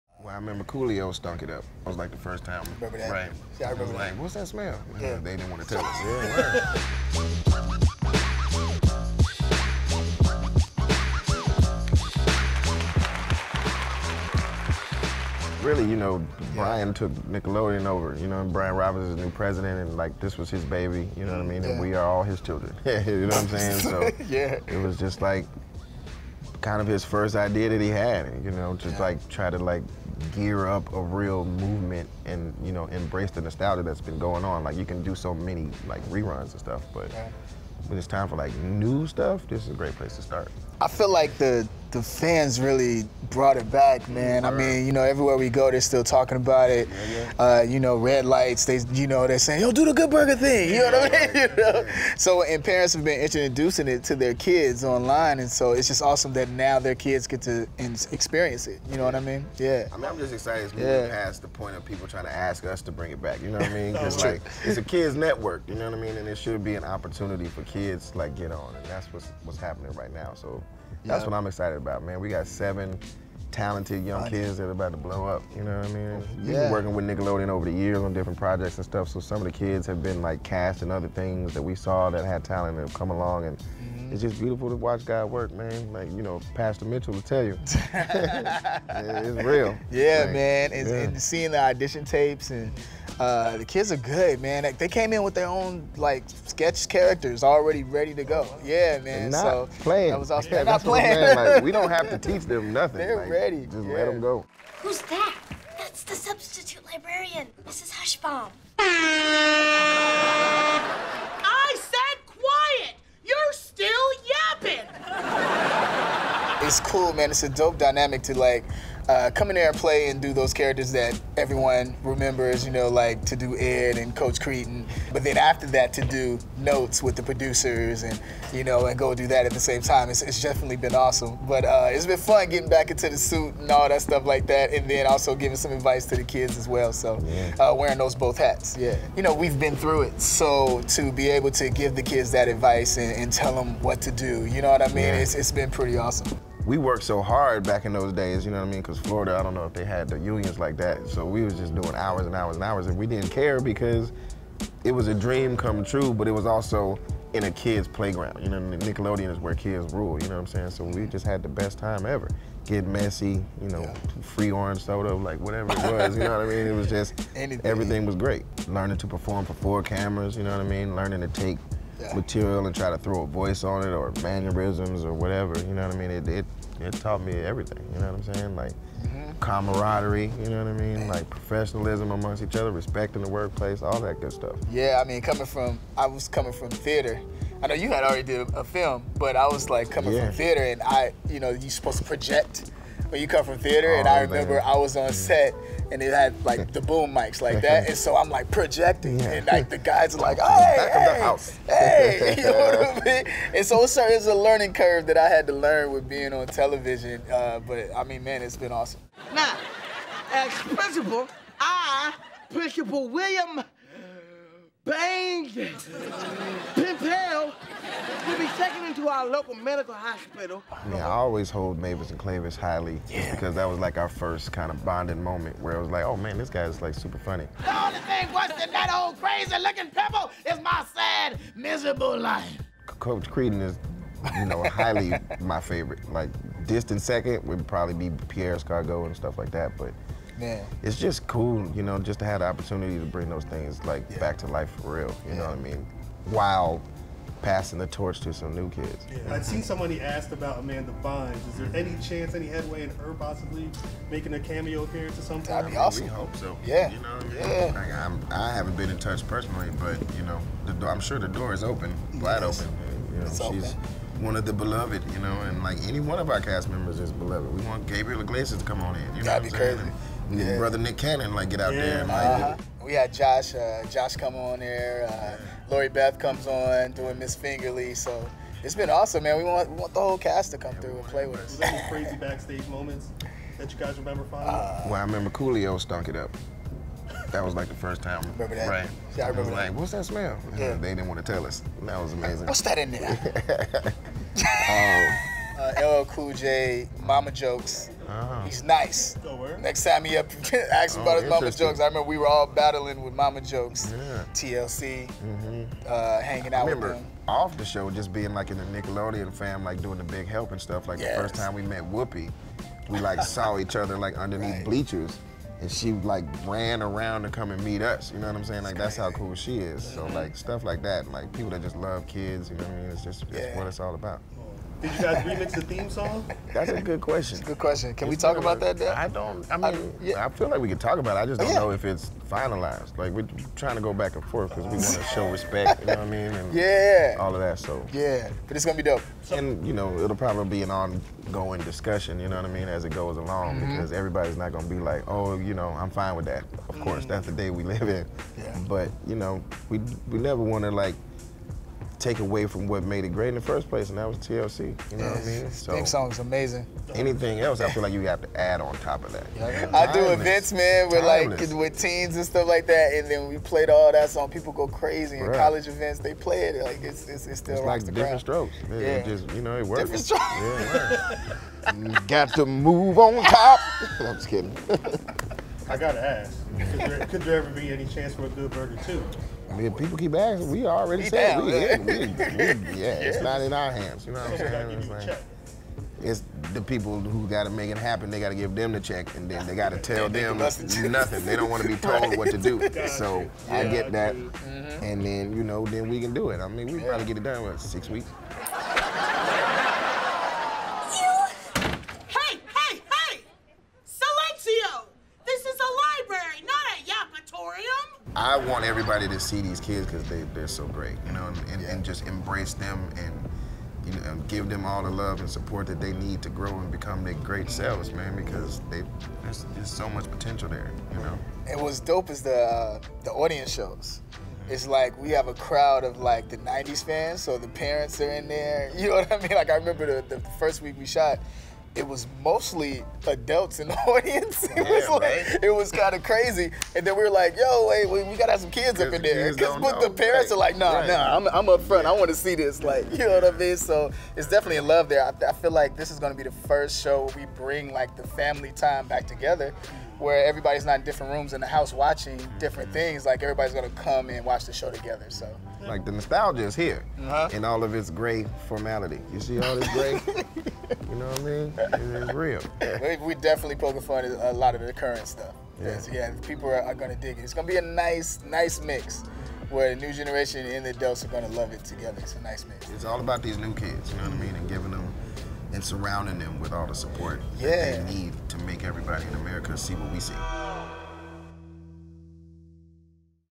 Kenan and Kel Talk 'All That' Reboot and 'Good Burger 2'
We caught up with comedy legends Kenan Thompson and Kel Mitchell to talk all things "All That" and Good Burger 2.